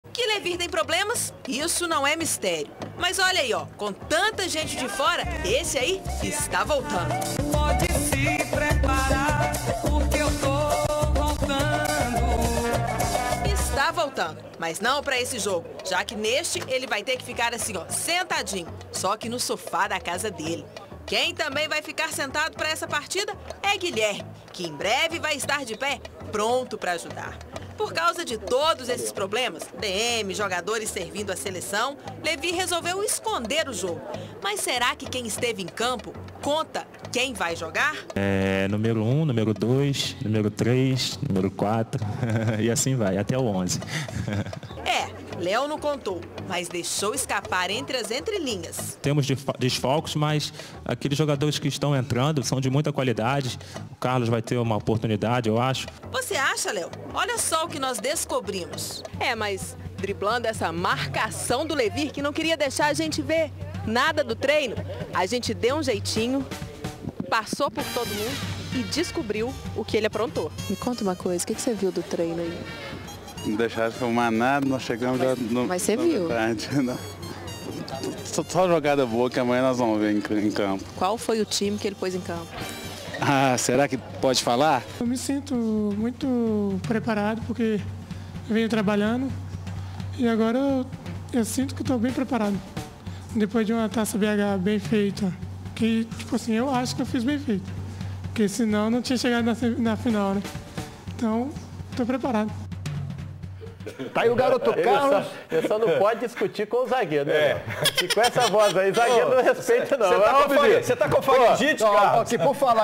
Jovem Carlos ganha chance no time titular do Atlético-MG
O treino de quarta-feira, na Cidade  do Galo, teve Carlos na equipe titular. O jovem atacante aproveitará o alto número de desfalques do Atlético para ganhar uma chance contra o Palmeiras, pela Copa do Brasil, no Independência.

Carlos deve compor o lado esquerdo do ataque atleticano. "Levir disse para jogar leve, pela esquerda, caindo pelas pontas. Jogar tranquilo", disse o jovem em entrevista coletiva.

Levir Culpi armou o time no treino com Victor, Alex Silva, Leonardo Silva, Jemerson e Pedro Botelho; Leandro Donizete, Rafael Carioca, Luan, Dátolo e Carlos; Jô.